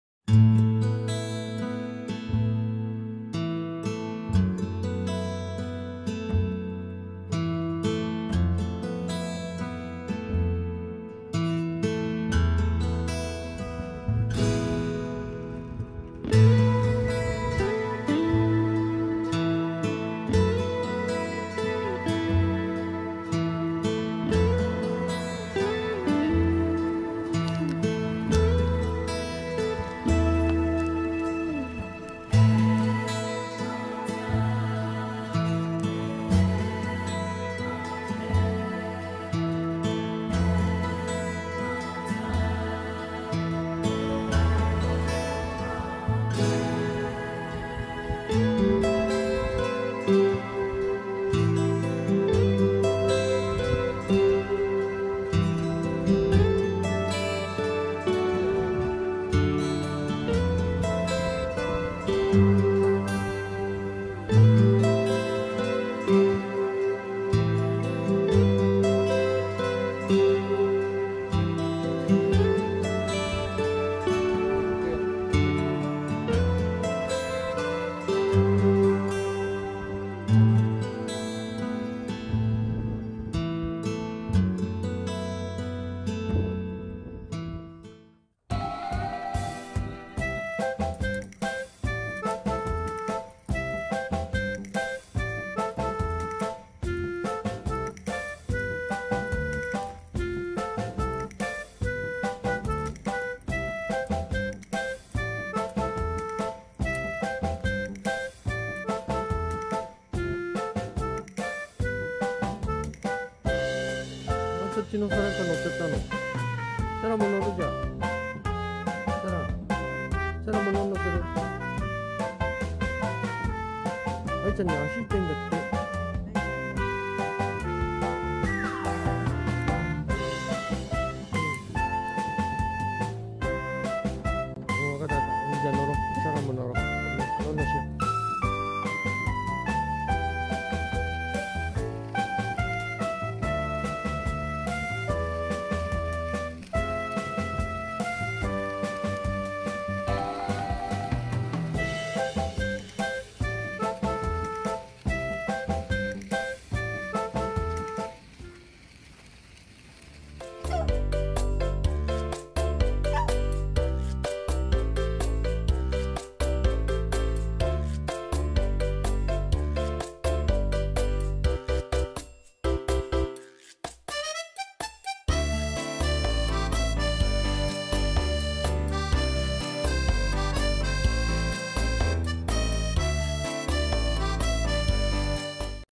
【ビーグル犬】わんこたちと夜さんぽ❗ 今宵何かが……🤔⁉️
【サランとあい】最近はわんこたちと夜さんぽするのが日課になりました❗

いつものおさんぽコースまでカートに乗って行くサランちゃん あいちゃんは歩いて行きます

おさんぽコースに着いてクンクンに夢中のサランちゃん

あいちゃんは夕方のドッグランで走って足🐾が痛いみたい…

🐶 あれ⁉️あいちゃんが… ママに懸命に訴えていたサランちゃんです❗